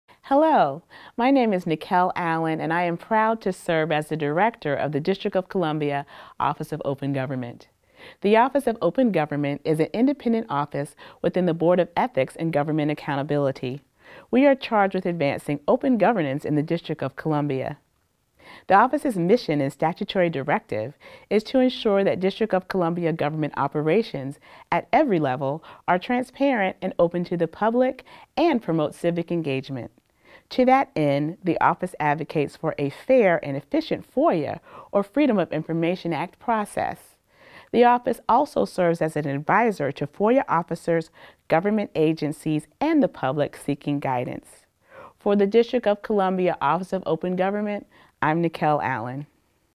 DC Office of Open Government PSA by Director Niquelle Allen
This video is introduces the DC Office of Open Government and features Director Niquelle Allen, Esq., who heads the DC Office of Open Government.

*Filmed at the DC Bar Production Studio*